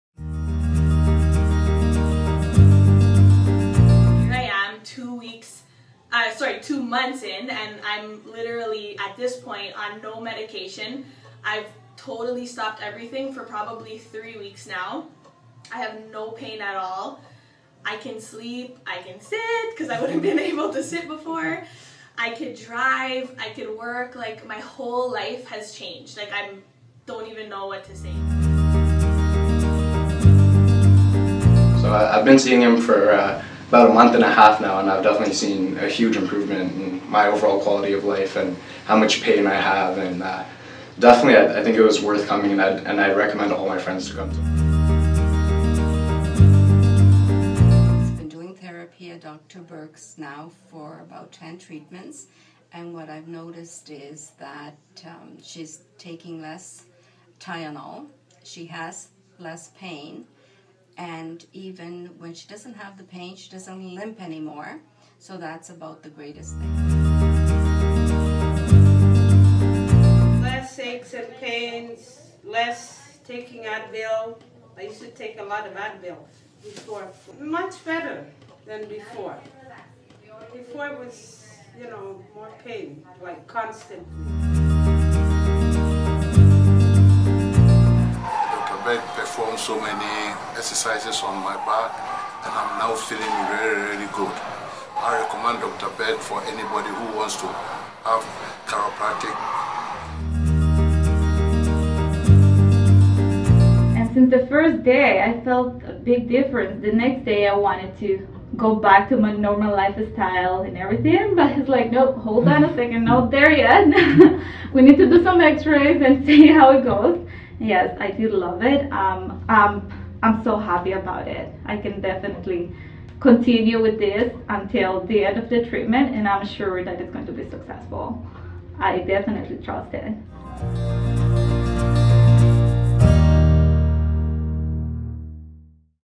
Our Amazing Patient Testimonials
Hear what our amazing patients have to say about their experience at Atlantis Wellness Center!